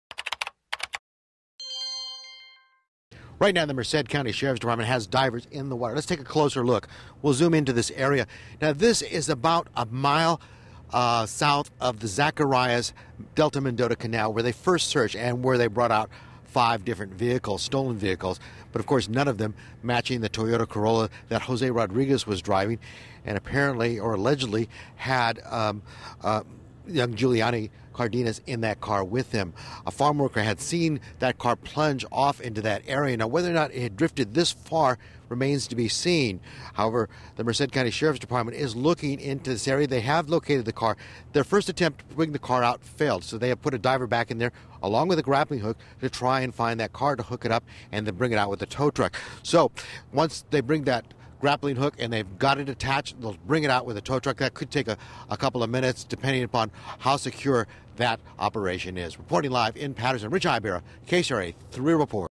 Crews Prepare For Car Recovery At Canal
Crews prepared Tuesday to pull another car out of canal waters in the search for missing 4-year-old Juliani Cardenas.